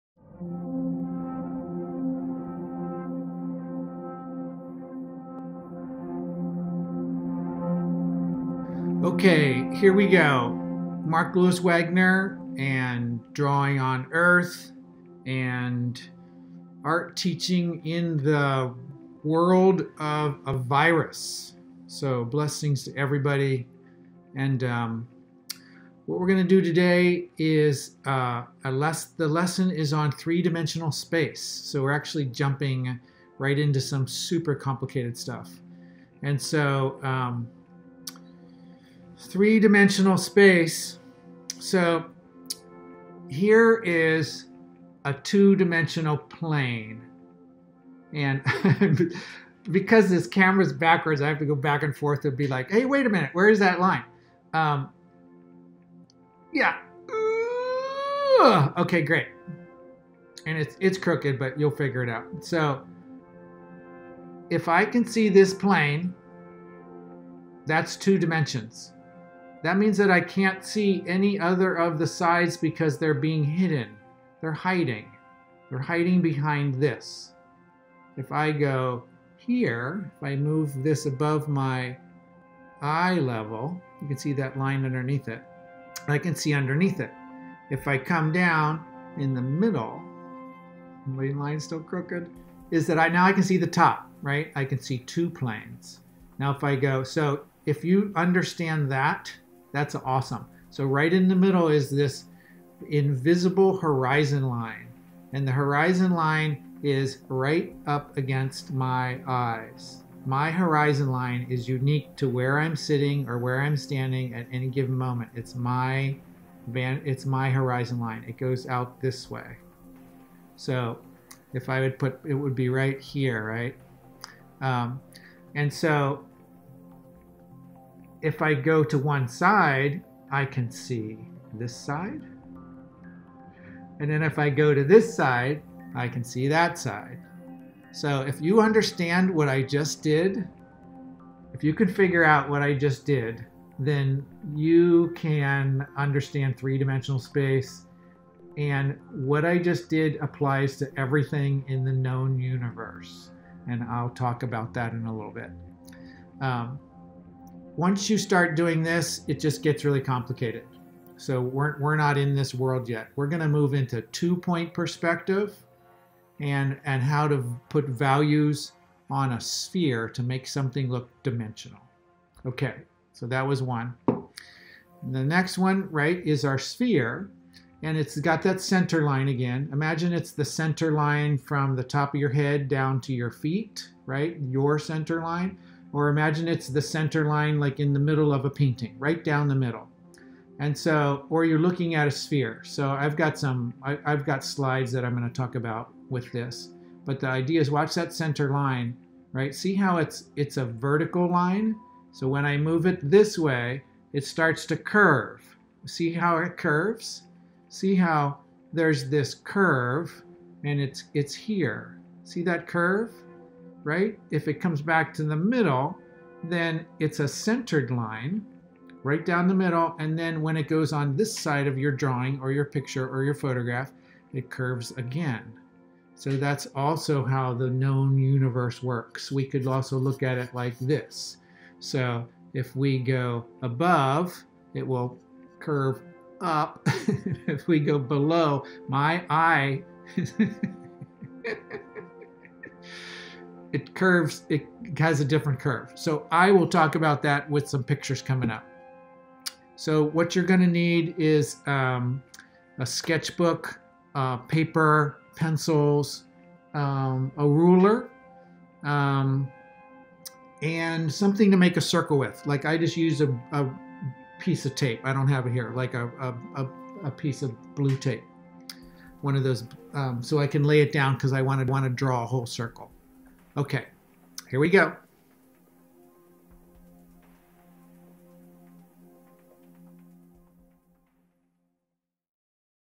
Drawing 3D (1of4) - Cube, Sphere, Perspective with Mark Lewis Wagner.
Drawing Three Dimensions (1of4) - Cube, Sphere, Perspective. For kids and adults. Mark Lewis Wagner and his nonprofit Drawing on Earth are producing a series of Art and Creativity classes for online educational learning.